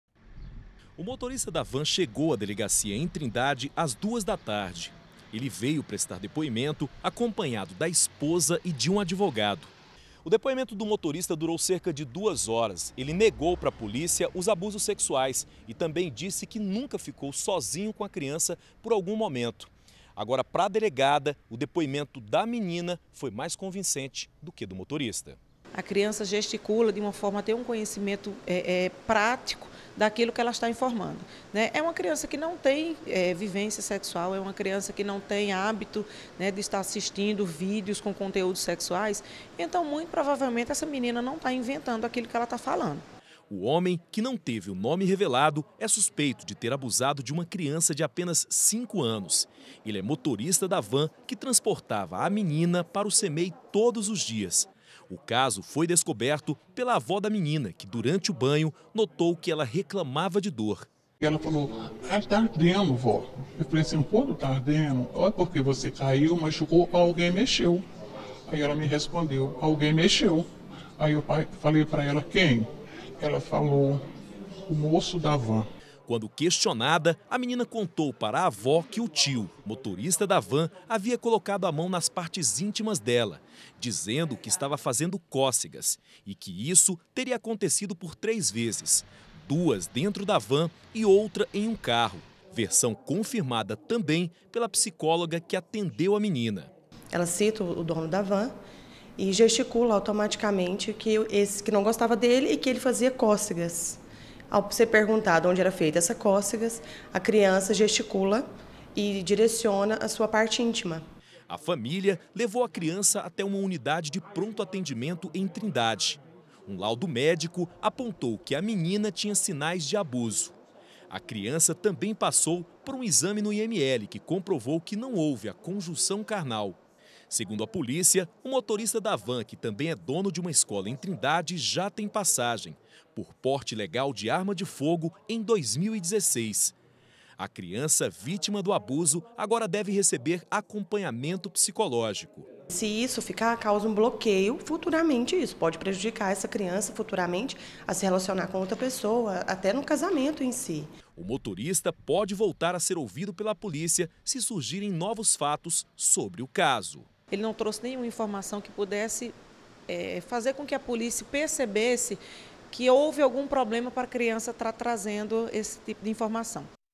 ABUSO SEXUAL: MOTORISTA DE VAN ESCOLAR NEGA CRIME
São sete e dez e o Goiás Record já está no ar. A gente começa mostrando que prestou depoimento hoje à tarde o motorista de van escolar, suspeito de abusar sexualmente de uma criança de 5 anos ele negou o crime, mas não convenceu a polícia. Já a criança foi bastante convincente ao falar do abuso.